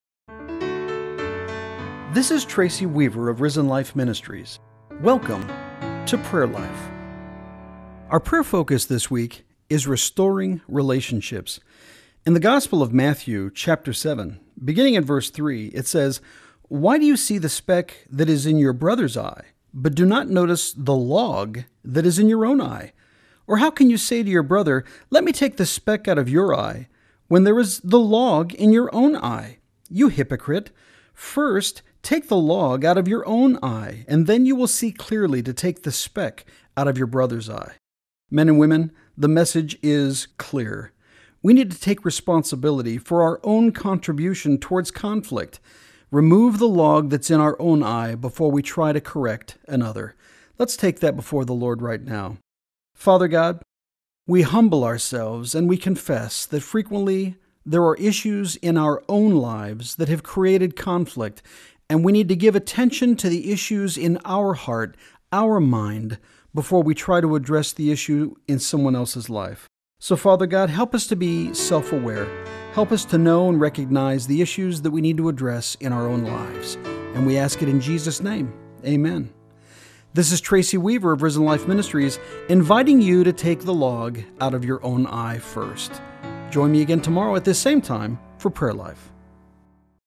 PrayerLife October 17 2012 Remove The Log
PrayerLife Focus: Remove The Log
Jesus was a master painter of word pictures.  Let's consider and pray about his humorous and insightful counsel for addressing relational conflicts.  Jesus said, "Why do you look at the speck of sawdust in your brother's eye and pay no attention to the plank in your own eye?  How can you say to your brother, `Brother, let me take the speck out of your eye,' when you yourself fail to see the plank in your own eye?  You hypocrite, first take the plank out of your eye, and then you will see clearly to remove the speck from your brother's eye" (Luke 6:41-42).